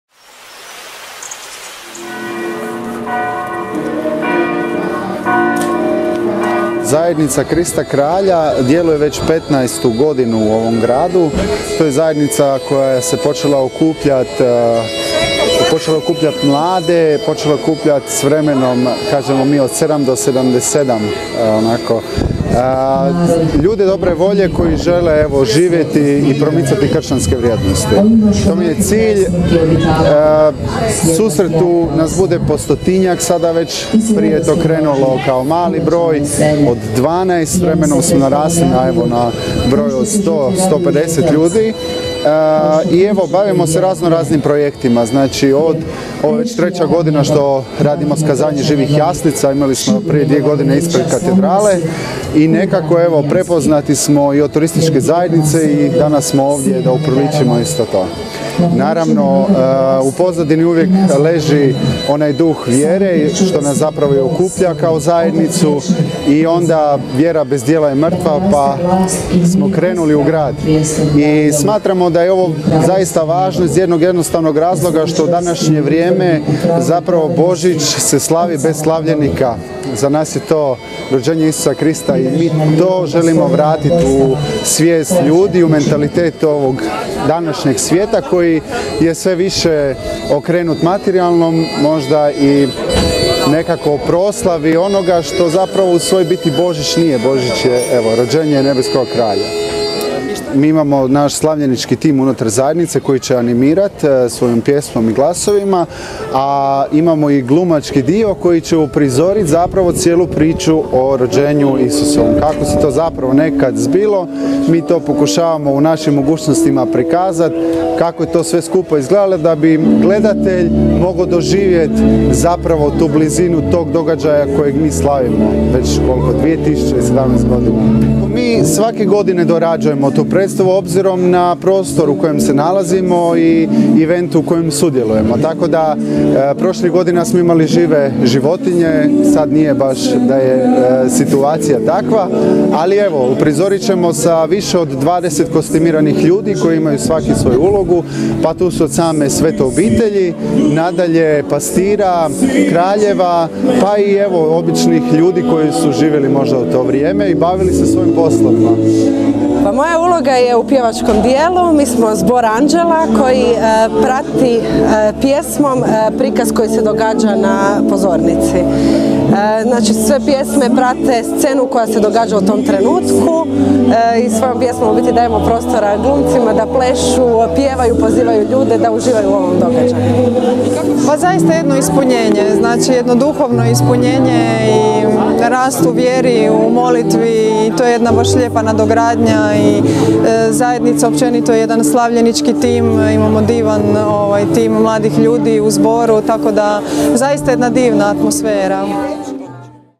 MojaRijeka.hr - Žive jaslice na Gradini
U nedjelju 10. prosinca, na Gradini, održalo se glazbeno-scenski uprizorenje živih jaslica u izvedbi Zajednice Krista Kralja Udruge Desetina iz Rijeke. U nastupu je sudjelovalo preko 20 kostimiranih članova koji na jedinstven način pričaju priču o Kristovom rođenju uz anđele i Betlehemsku zvijezdu, štalicu s Marijom, Josipom i Isusom u jaslama te pastirima i tri kralja.